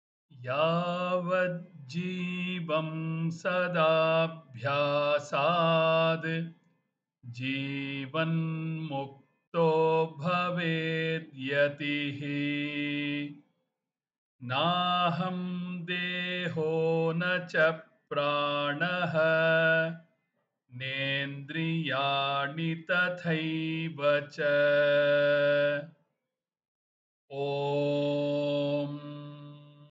Brahmanuchintanam - Verse 21
This is a very short treatise wherein Sri Sankara Bhagavatpadacharya has condensed the main doctrine of Advaita and also the practical means to be adopted to realise it here and now.

Introduction extracted from Shri Jnanananda Swamigal's translation of Shri Sankara's Brahmanuchintanam.